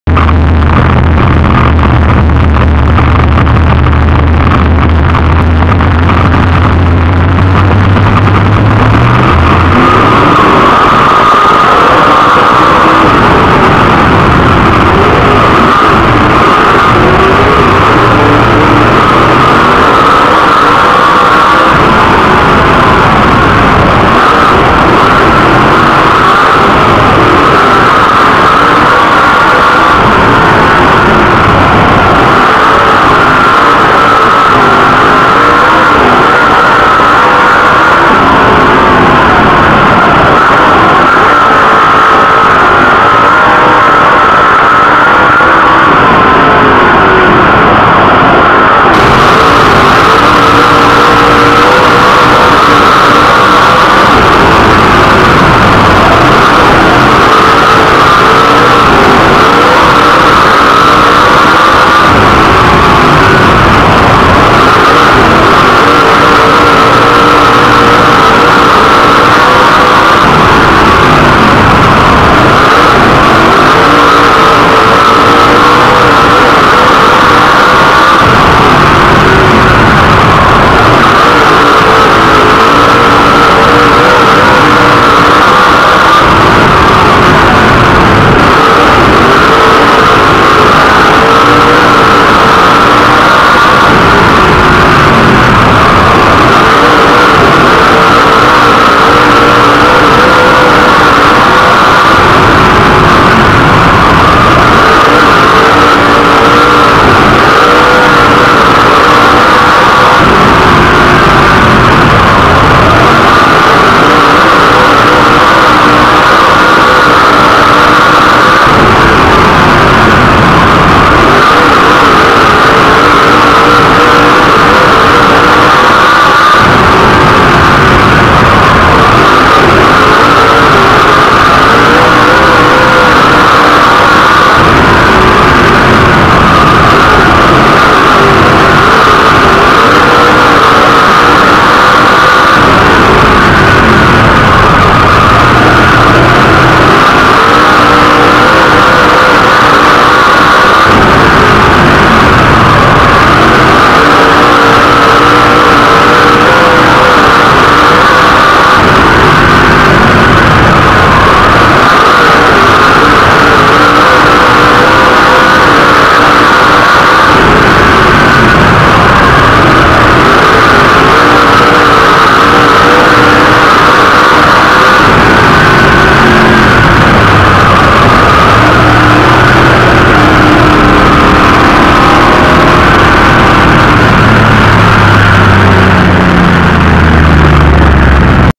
#5 Brad Omps, Heat Race 7-3-10, Winchester Speedway
#5 Brad Omps, Heat Race 7-3-10, Winchester Speedway, in-car camera onboard, dirt track super late model